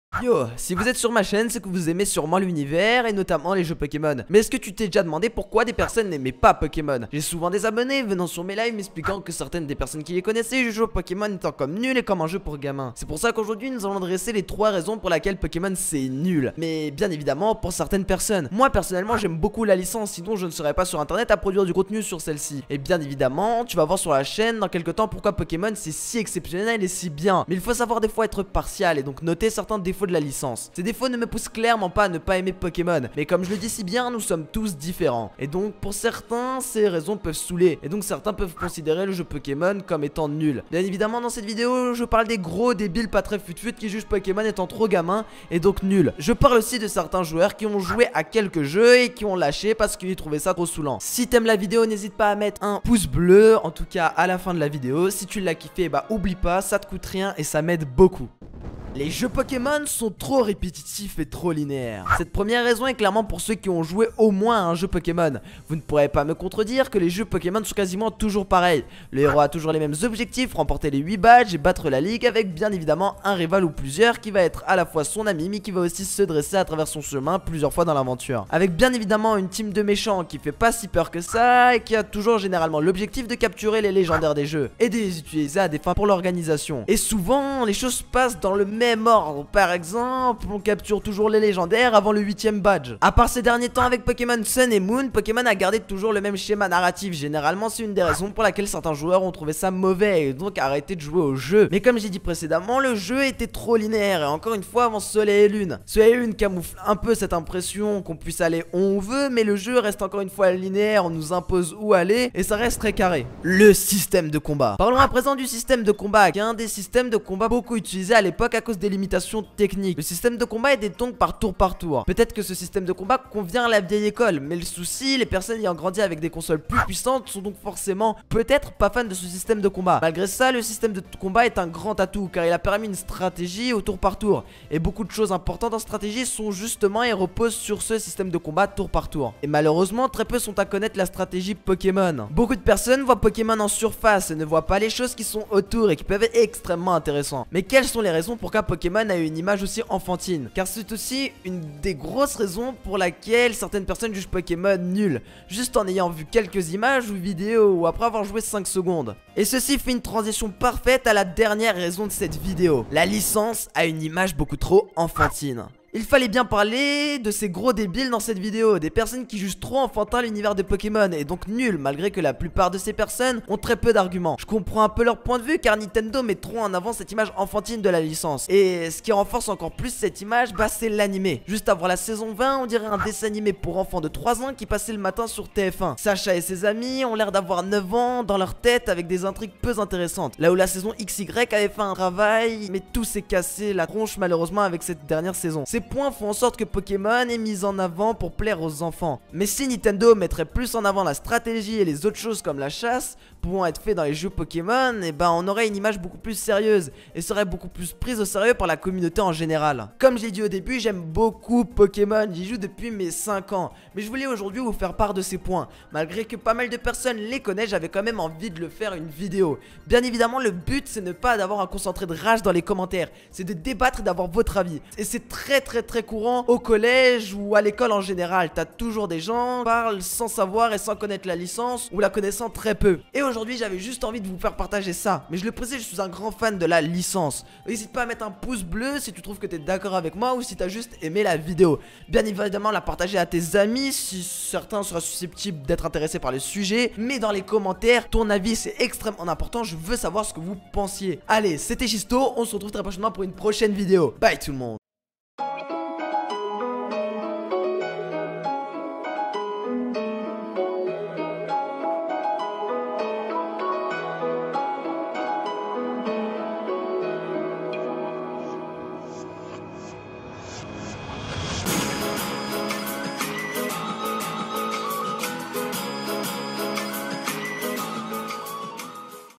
POKEMON A DE GROS DEFAUTS !-LA CRITIQUE #01
Hey salut tous le monde c'est  shisto aujourd’hui vidéo ou je vous  explique pourquoi pokemon serait nul pour certaine personne ^^ !